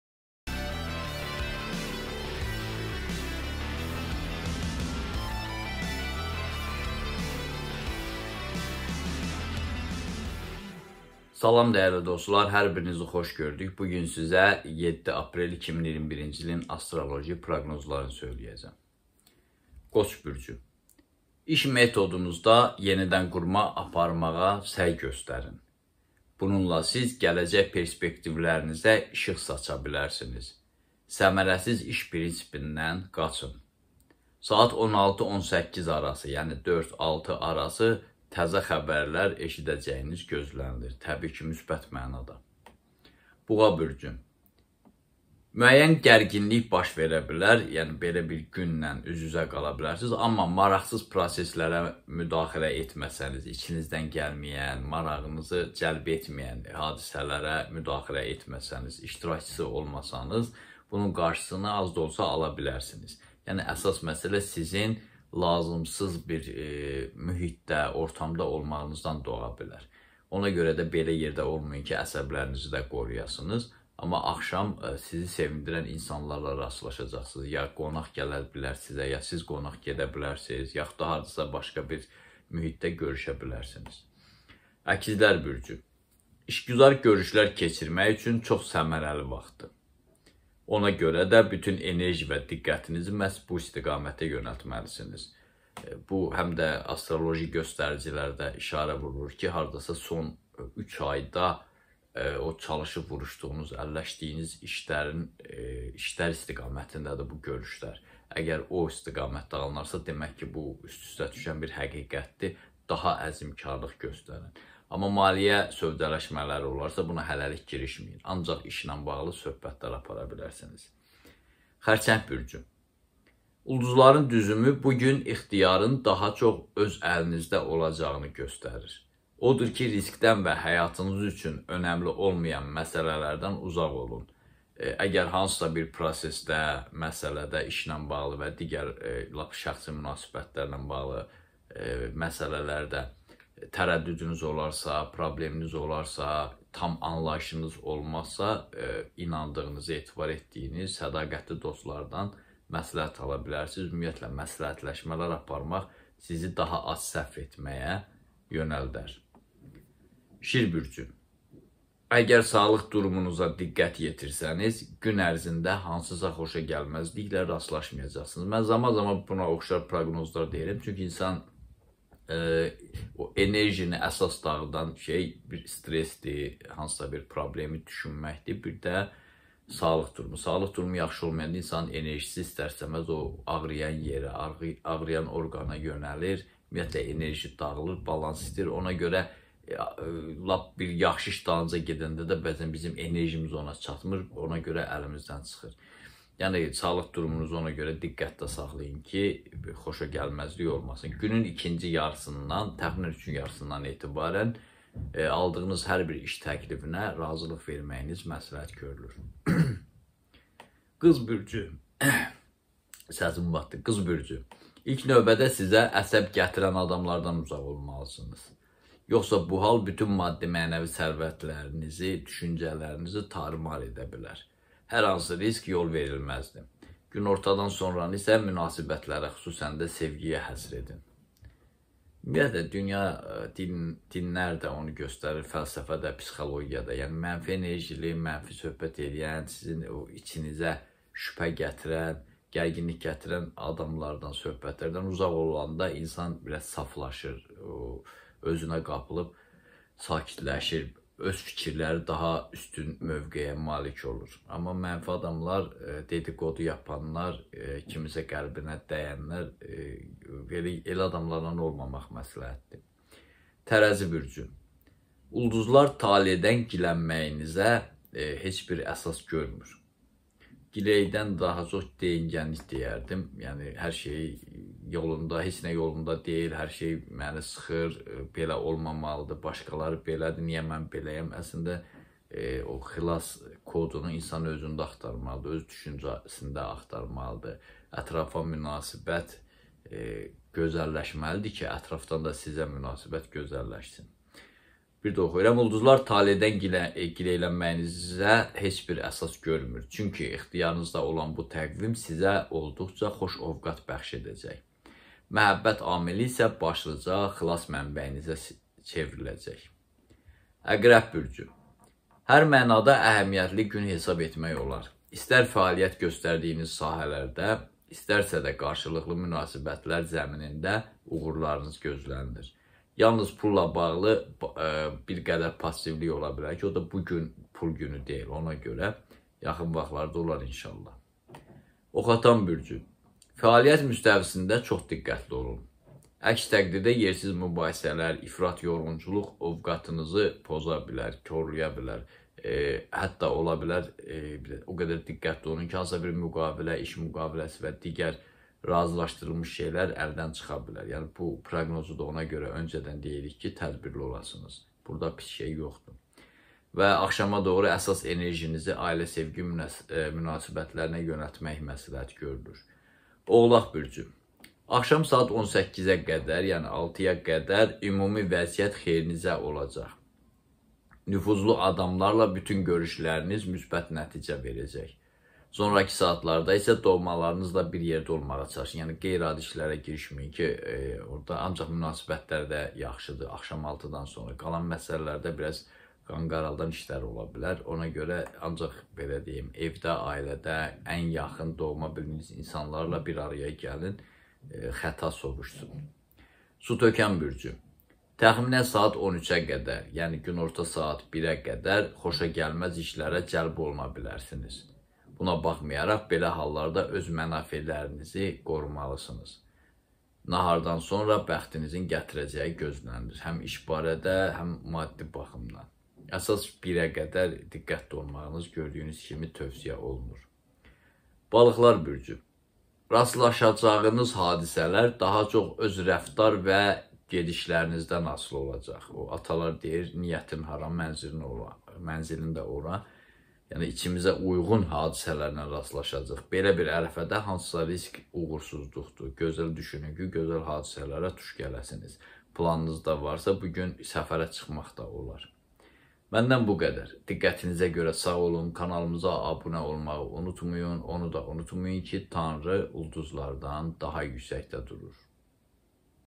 BÜRCLƏR – 7 APREL (canlı)
Astroloq Səbuhi Rəhimlinin təqdimatında bürclər üzrə 7 APREL üçün astroloji proqnoz (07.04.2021)